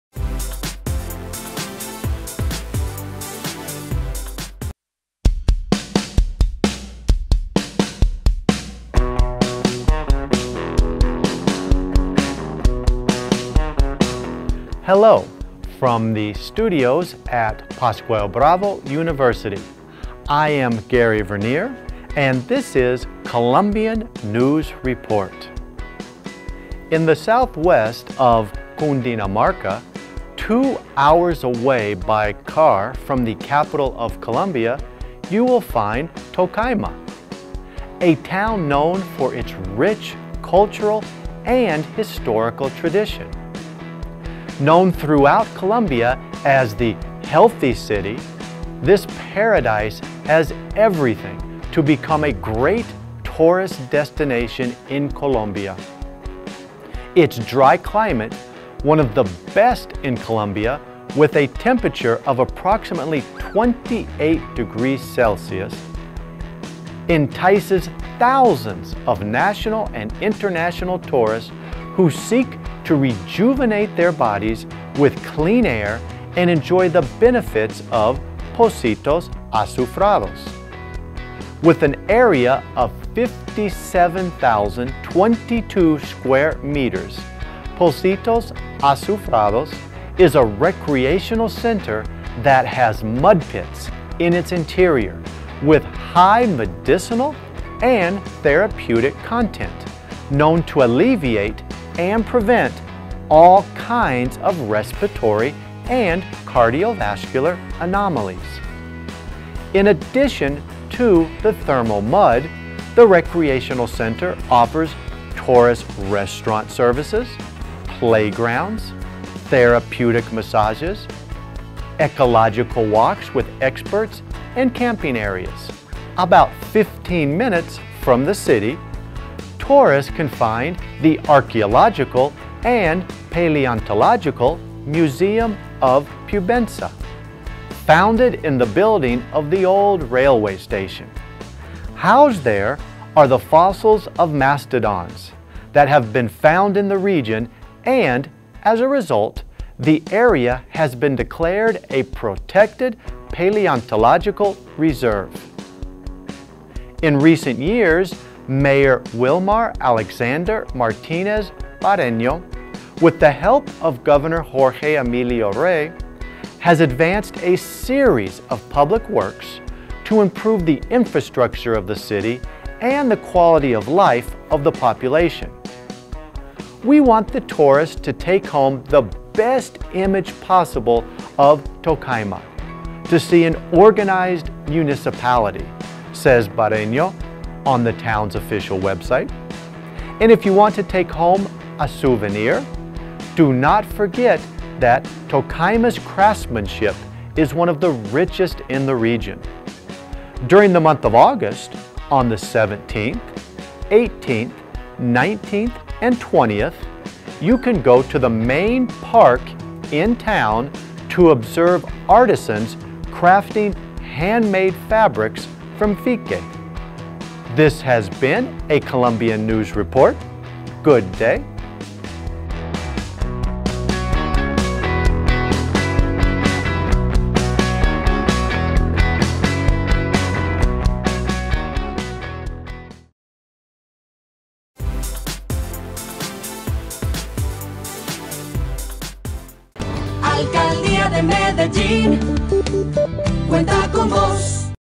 Virtual English Level 2 Tocaima, Colombia
COLOMBIAN NEWS REPORT
In the southwest of Cundinamarca, two hours away by car from the capital of Colombia, you will find  Tocaima, a town known for its rich cultural and historical tradition. Known throughout Colombia as the "healthy city", this paradise has everything to become a great  tourist destination in Colombia.

Coordinador

Soporte Tecnológico